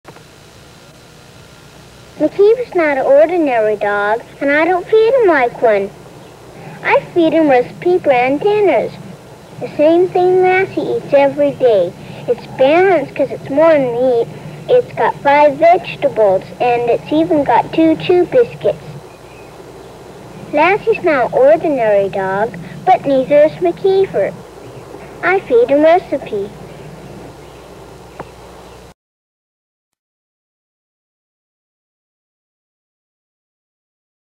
Recipe Dog Food Commercial (Lassie, Early 1970s)
"The balanced dinner Lassie eats."
Recipe Hearty Meat Stew dog food commercial featuring Lassie and McKeifer. Spot aired in the early 1970s (likely 1970-71).